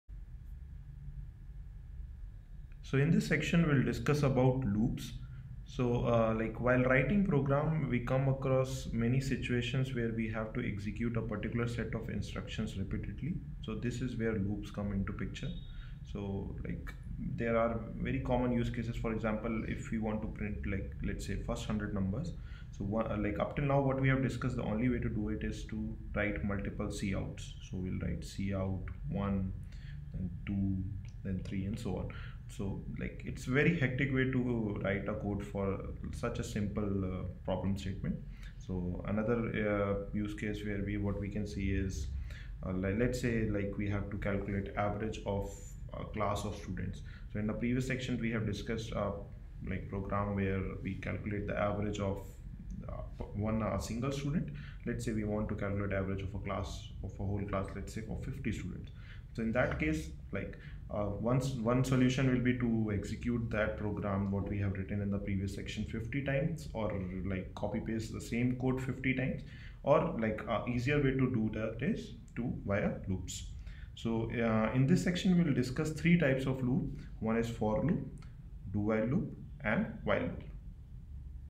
(1/14) Introduction to loops in C++ | Beginners
This is the third series of videos for people who want to learn c++ from the beginning.
In this tutorial we will see the introduction of loops in c++;